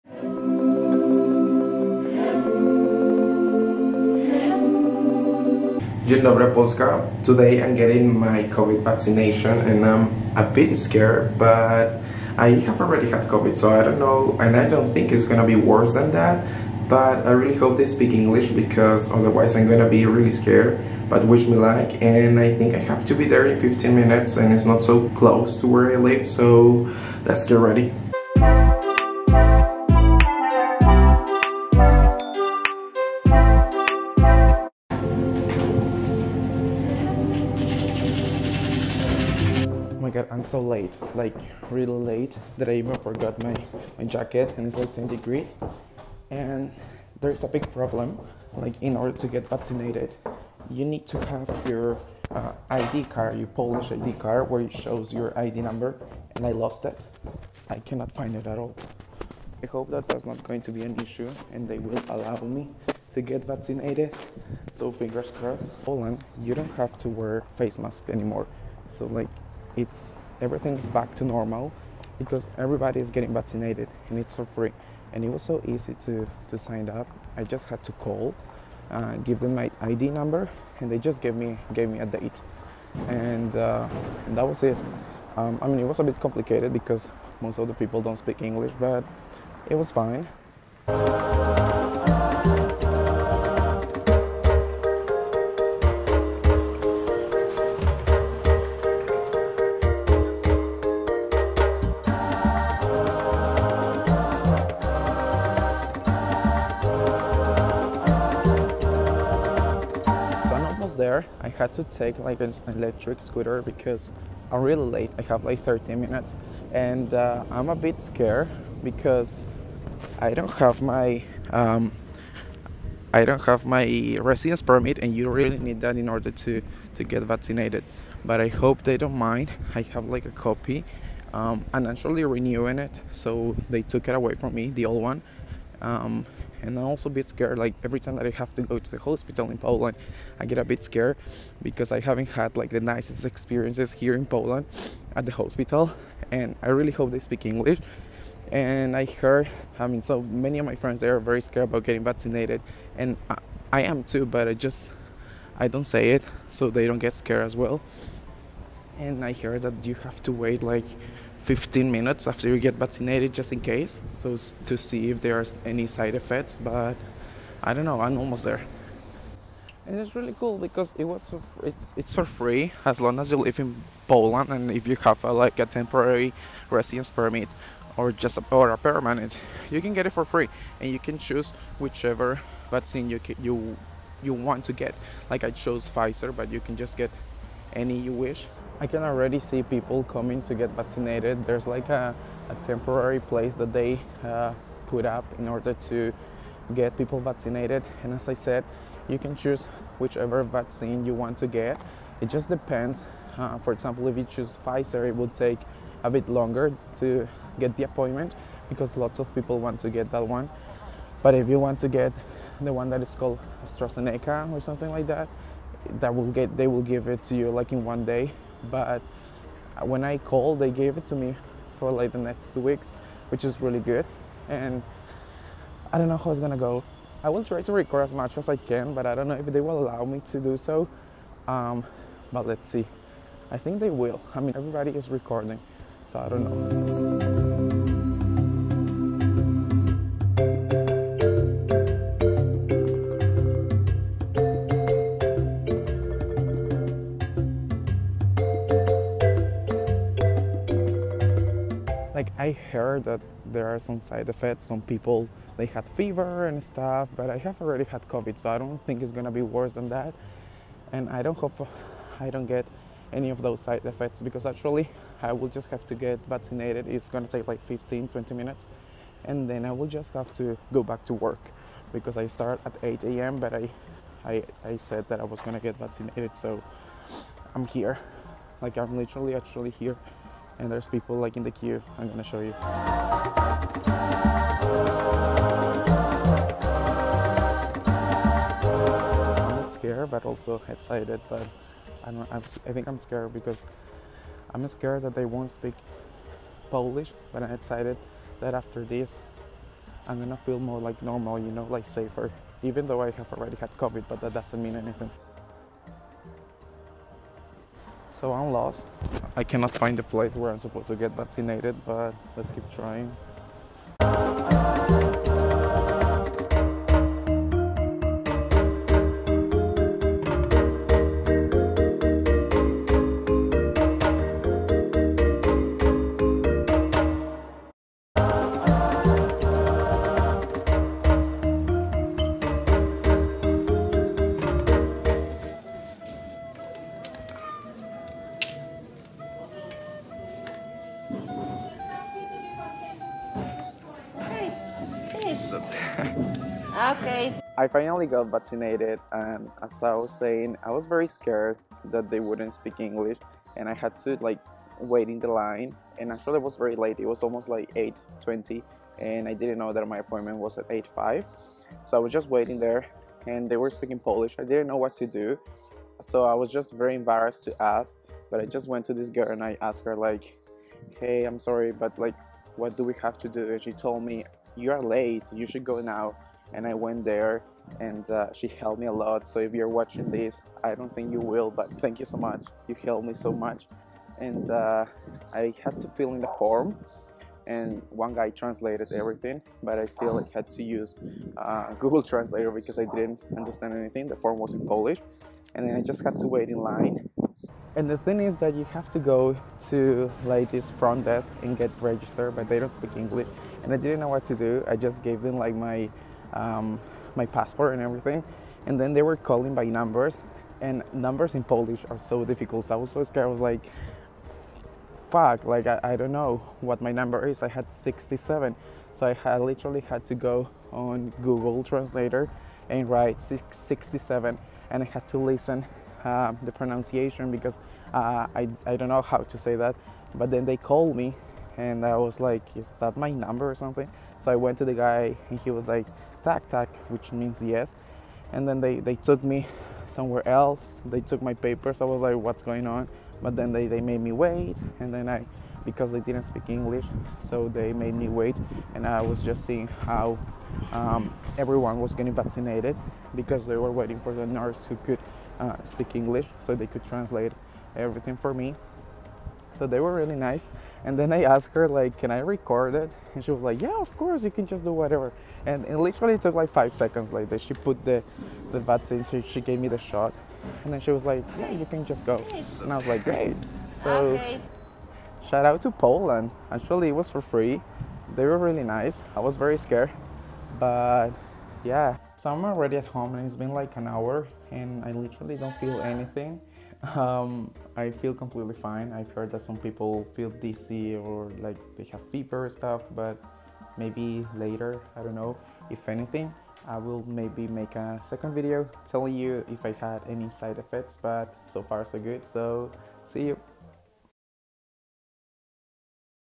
VLOG #2 GETTING VACCINATED IN POLAND ( COVID EDITION)
Hello Guys for today's first Vlog I wanted to show you how it is to get vaccinated in Poland and what the procedure looks like 😄I hope you enjoy it ☺️

¡Gracias por mirar y que disfrutes! 🍻